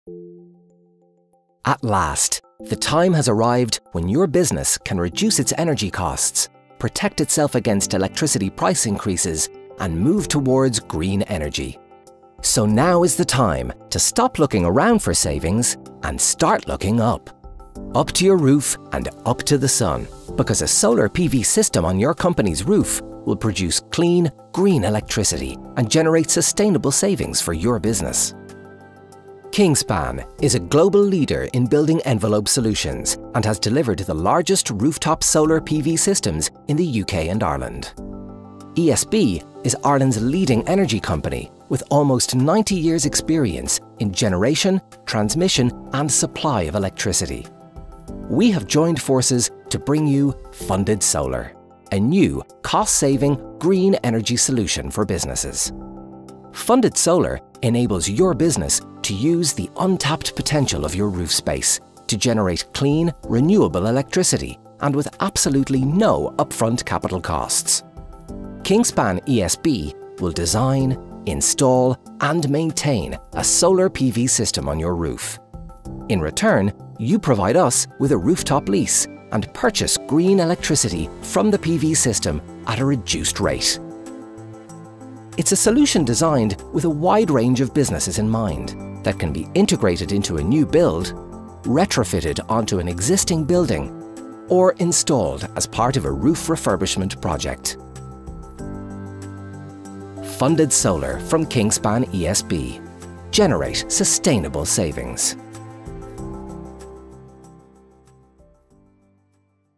Kingspan ESB Solar PV Joint Venture
Short video explaining new Solar PV Joint Venture between Kingspan and ESB